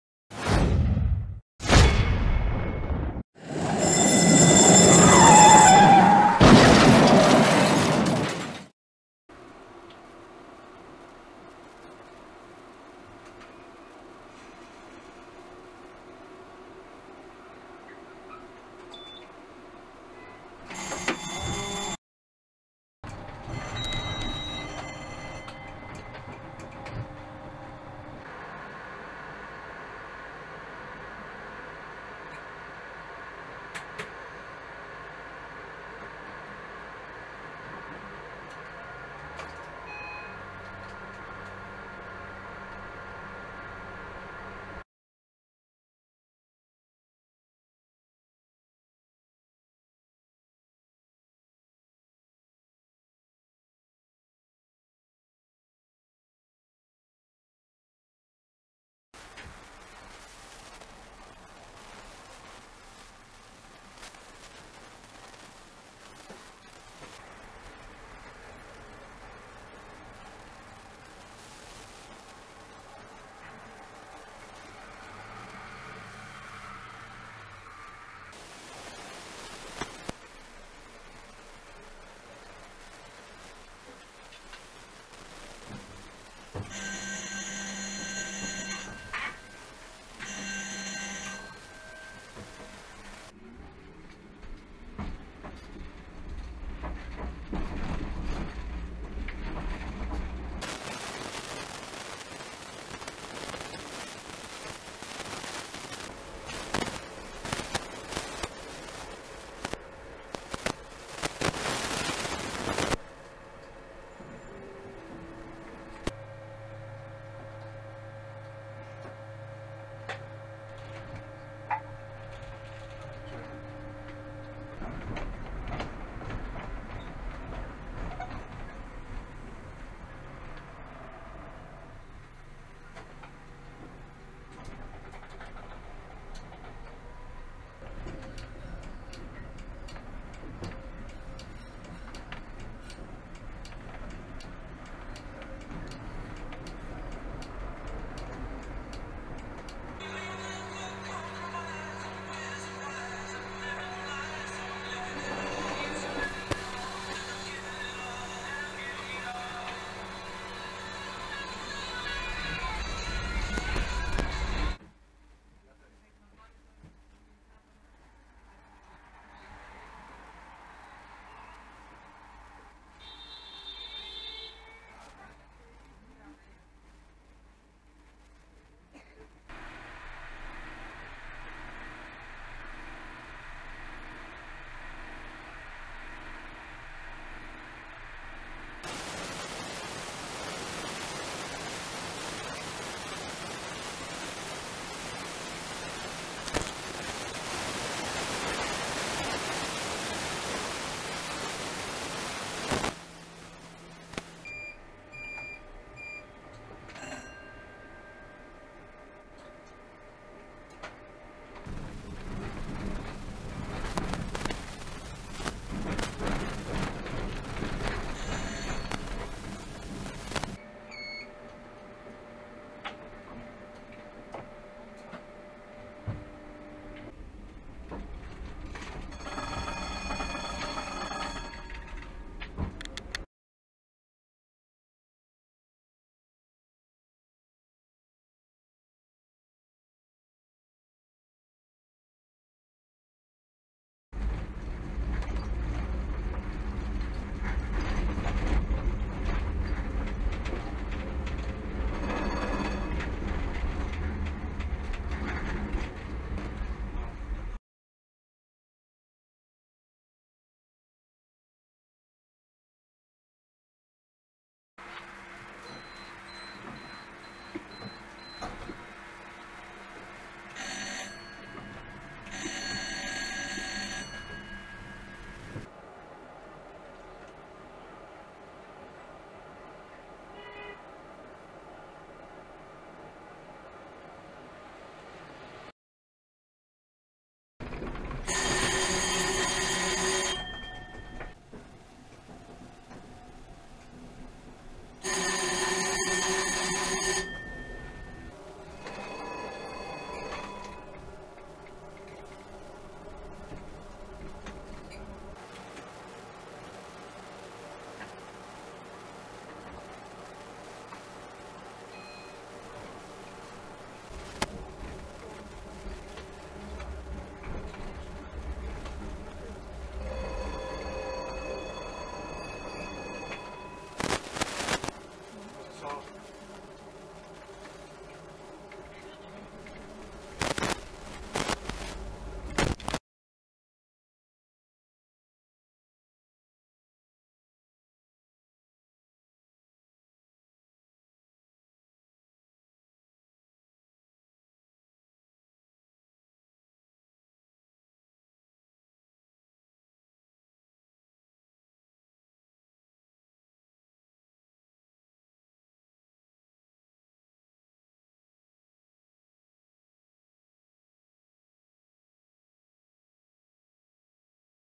#4 Piesi i ich zachowanie
Filmik zawiera NIEKTÓRE sytuacje, które zdarzają się codziennie na ulicach Szczecina. Jest on przestrogą dla kierowców oraz jakie sytuacje, możecie trafić.

Zgodnie z obietnicą, kolejna kompilacja zachowań pieszych.

⏺💬 Na  czeka Was ankieta związana z pierwszeństwie dla pieszych 🧐